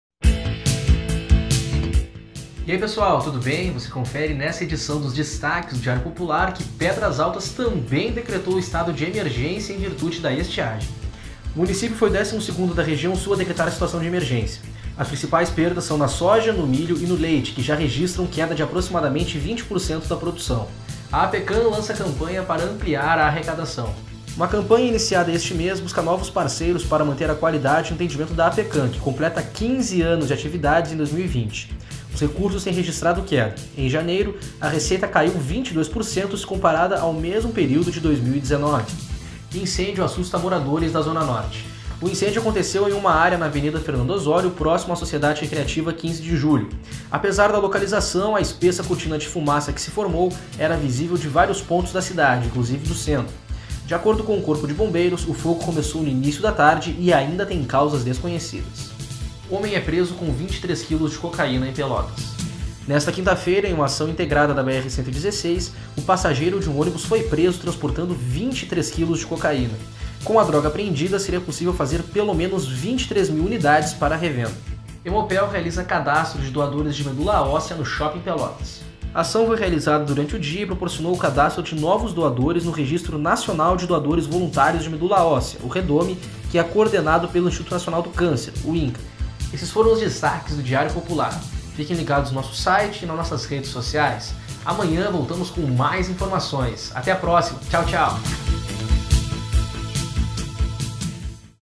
Destaques DP 27 02 2020
Destaques DP - Pedras Altas é o 12º município da Região Sul a decretar estado de emergência por causa da estiagem. Incêndio assusta moradores da Zona Norte de Pelotas. Aapecan lança campanha para ampliar a arrecadação. E tem ainda a prisão de um homem com 23 quilos de cocaína e o cadastro do Hemopel para novos doadores de medula óssea. Confira: